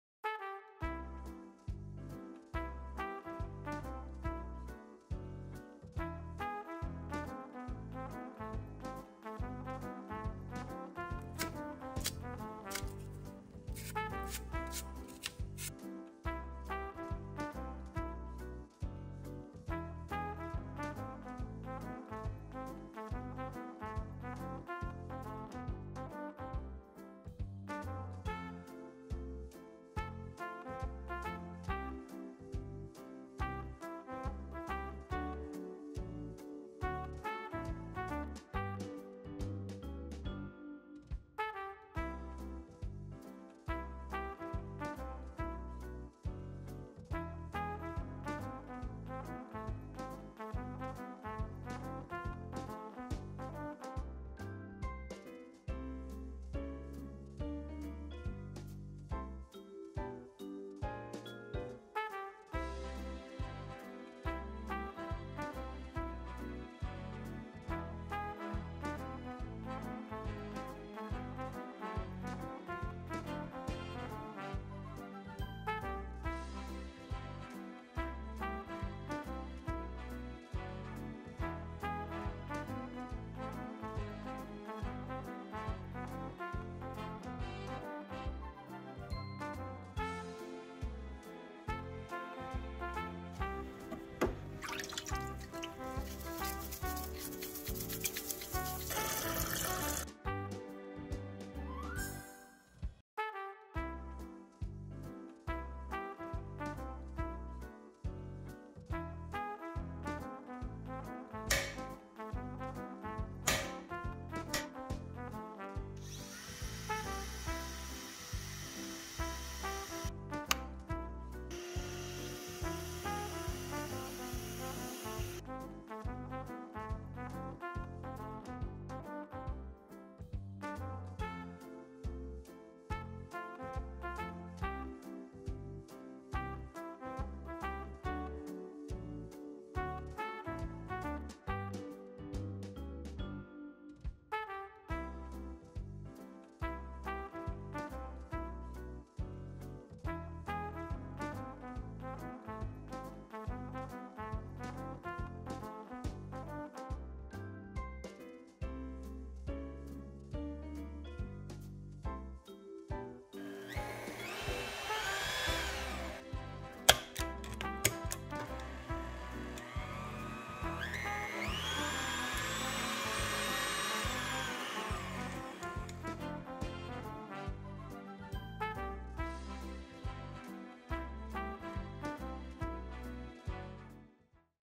How KitchenAid Mixers Are Professionally Deep Cleaned
Mr. Mixer specializes in the restoration and cleaning of KitchenAid and other brand mixers. For this piece, his client tried to take one apart and clean it themselves but weren't able to, so they brought it to him. He'll take out all the parts, remove all the built-up grease, clean the gears in a degreasing-solution tank, and put it all back together with new grease to help the gears run smoothly.